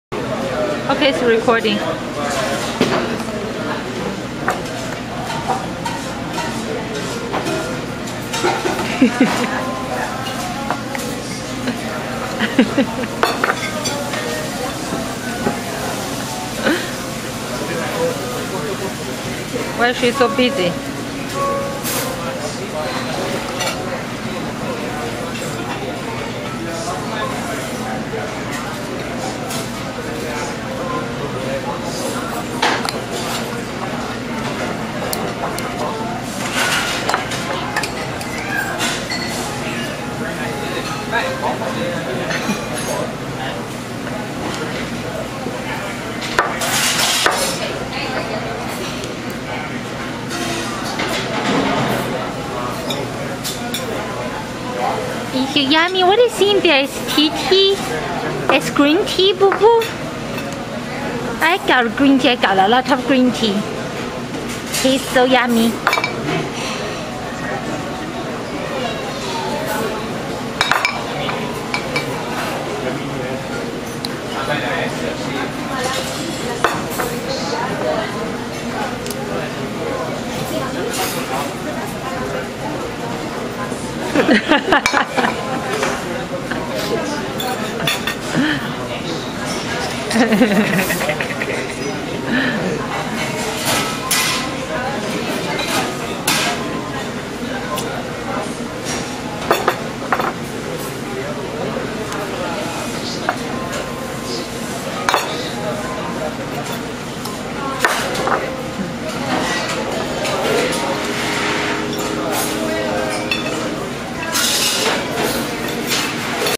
Attelia "drinking tea"
04/16/2011, in a Bertucci's restaurant, Attelia was pretending she was drinking tea from an empty  cup. She drank and drank. She drank a lot of tea!